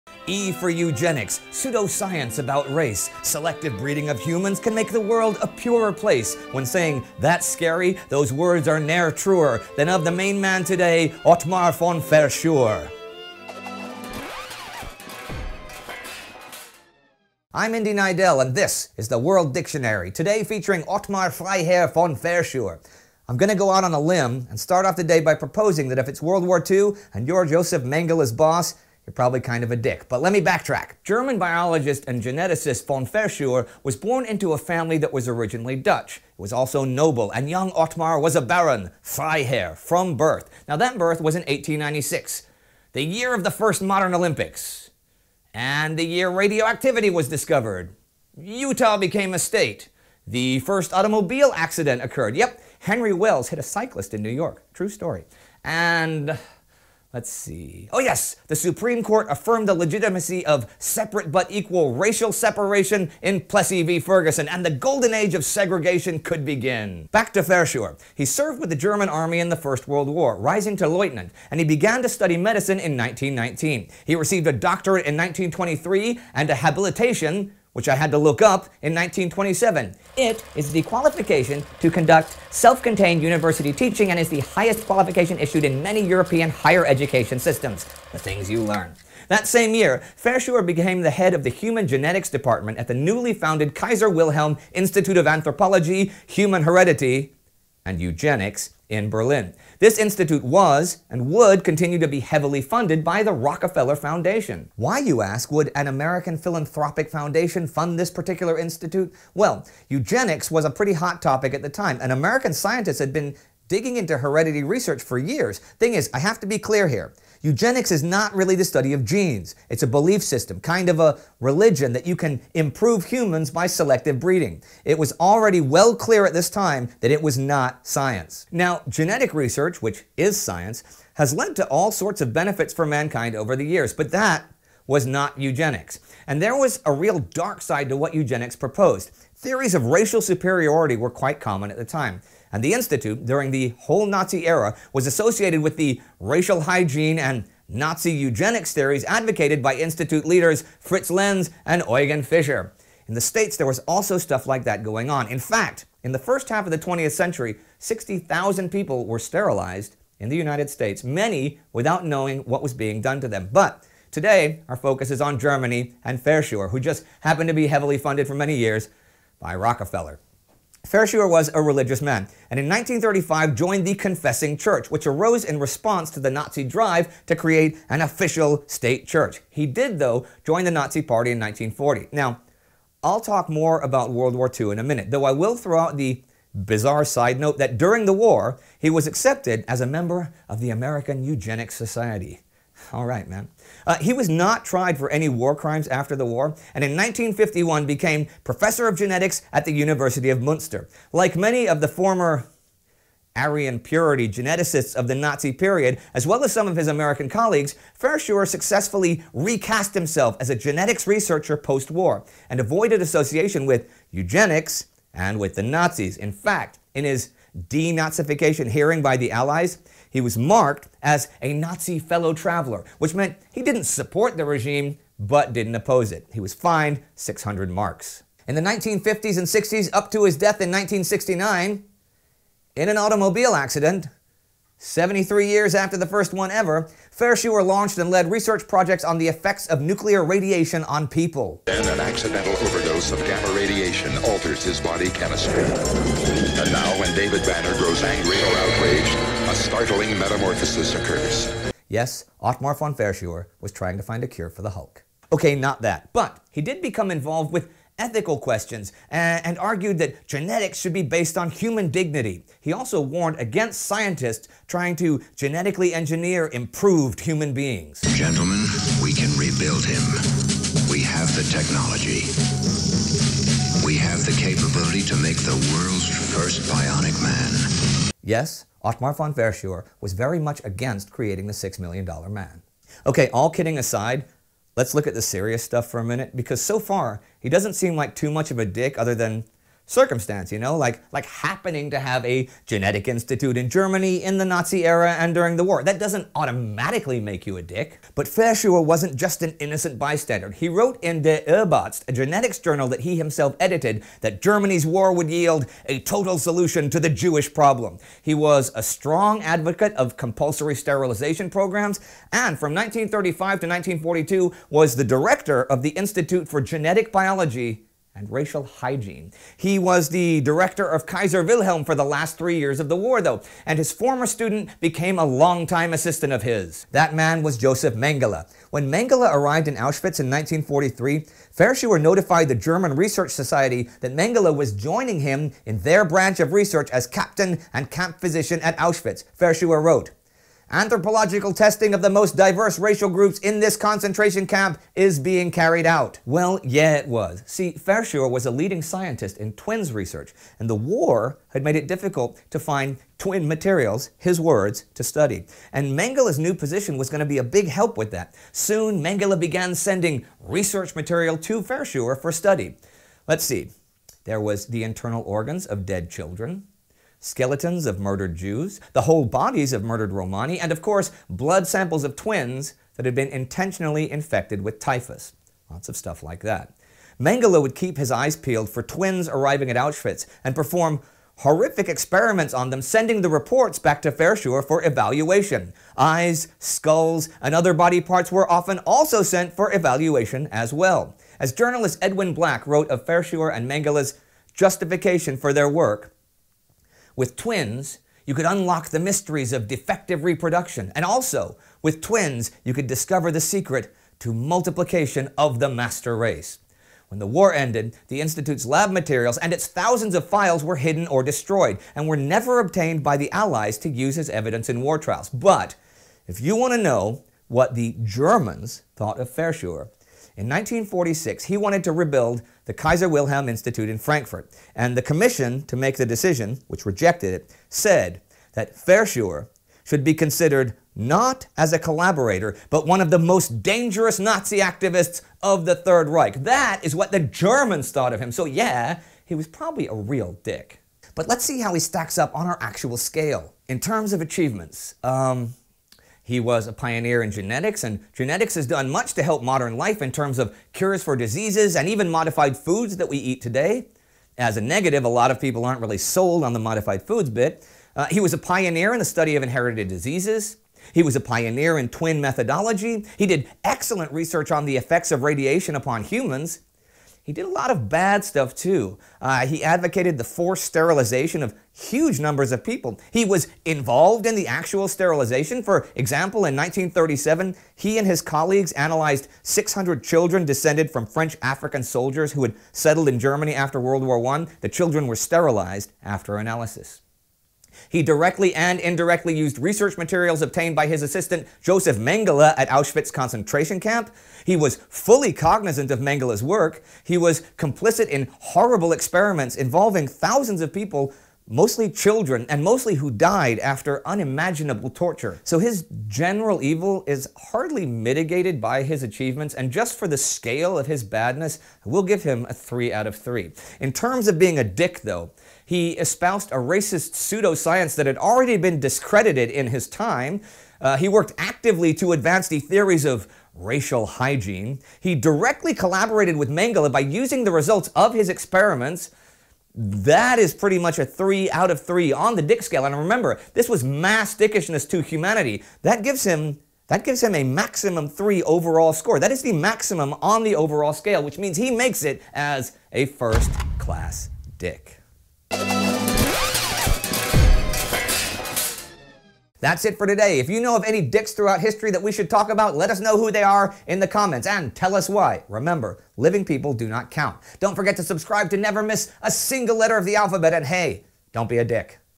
DicKtionary - E is for Eugenics - Otmar von Verschuer
E for eugenics, pseudo science about race,
Selective breeding of humans can make the world a purer place,
When saying, “that’s scary”, those words are ne’er truer,
Then of the main man today, Otmar von Verschuer.

Written and Hosted by: Indy Neidell
Based on a concept by Astrid Deinhard and Indy Neidell
Produced by: Spartacus Olsson
Executive Producers: Bodo Rittenauer, Astrid Deinhard, Indy Neidell, Spartacus Olsson
Camera by: Ryan Tebo
Edited by: Bastian Beißwenger

A TimeGhost format produced by OnLion Entertainment GmbH